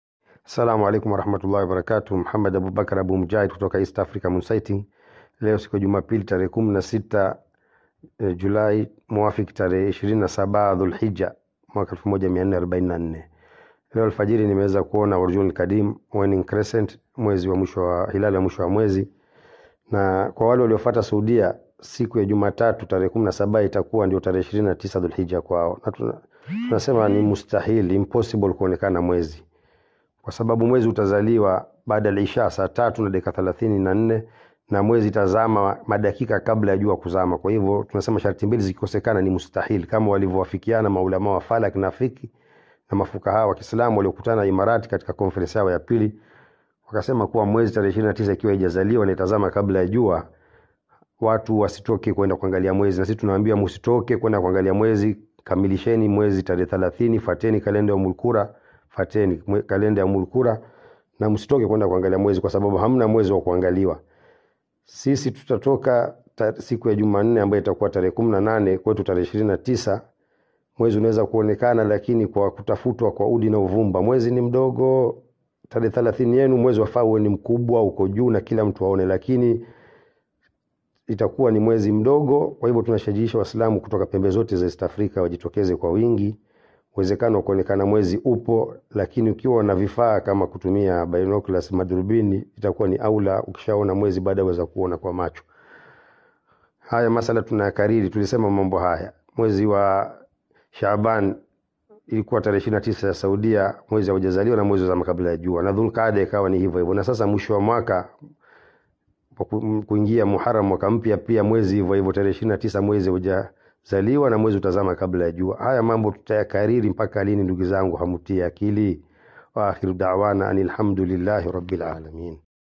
Hilal ya Muharram 1445 AH.
*Mwaka mpya Muharram ya 1445 AH.*

Tujitokeze Kwa wingi kutafuta hilal ya Muharram, _siku ya Tuesday 18th of July 2023 / 29 ya Dhul hijjah 1444 AH._
 *EAST AFRICA MOONSIGHTING*